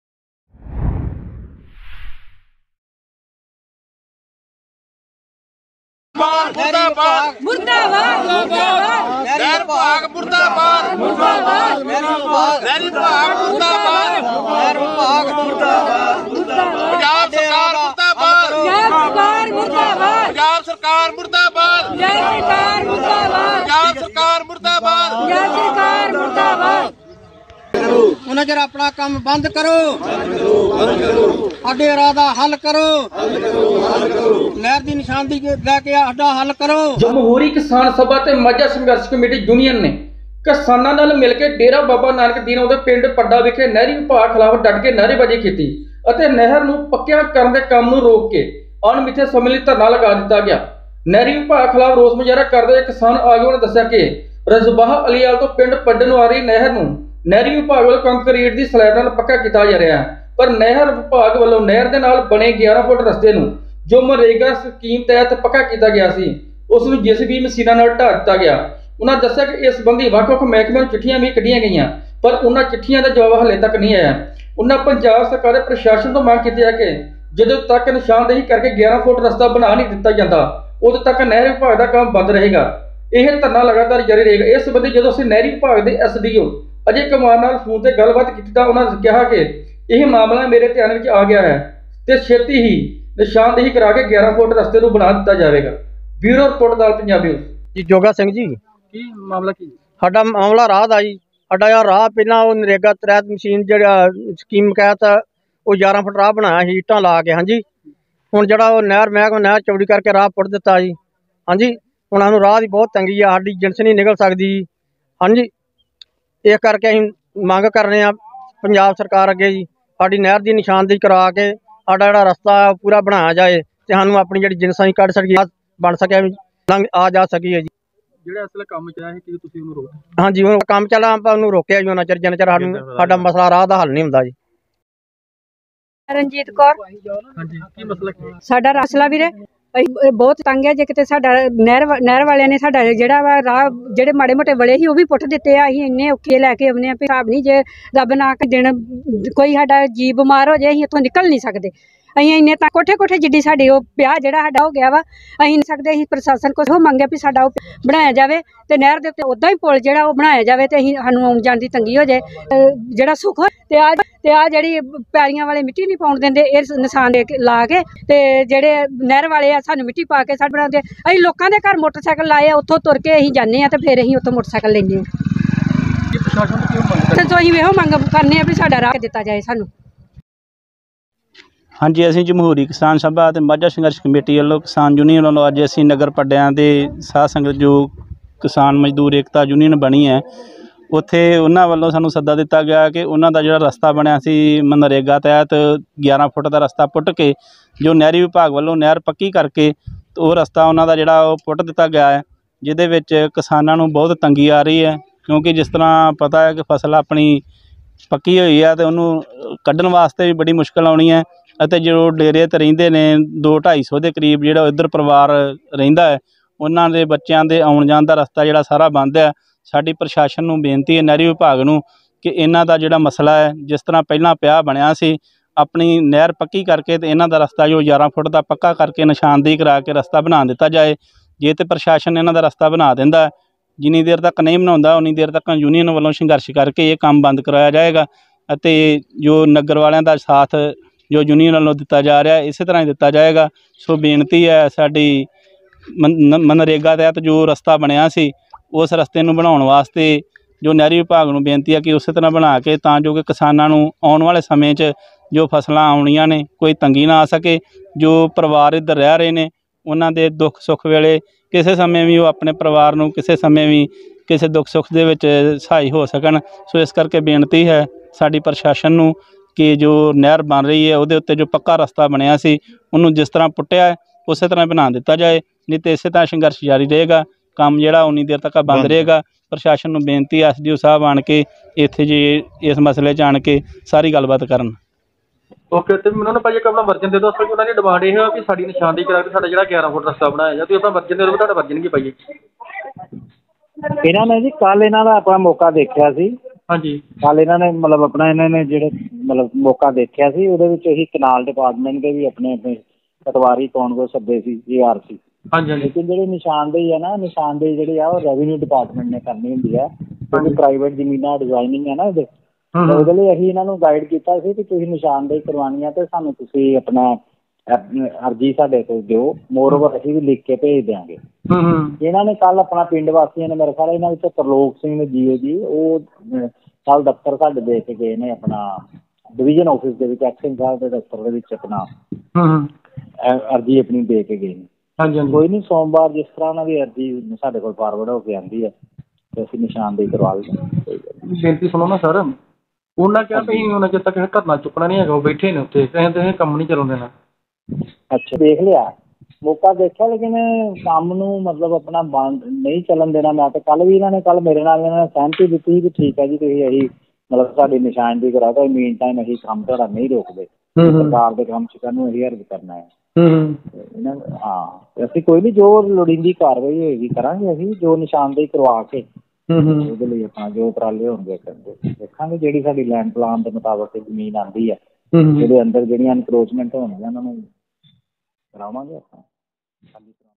Residents of village Pada in district Gurdaspur staged a protest against the canal department
ਤਹਿਸੀਲ ਡੇਰਾ ਬਾਬਾ ਨਾਨਕ ਅੰਦਰ ਪੈਂਦੇ ਪਿੰਡ ਪੱਡਾ ਨਿਵਾਸੀਆਂ ਨੇ ਨਹਿਰੀ ਵਿਭਾਗ ਖਿਲਾਫ ਕੀਤਾ ਰੋਸ ਮੁਜ਼ਾਹਰਾ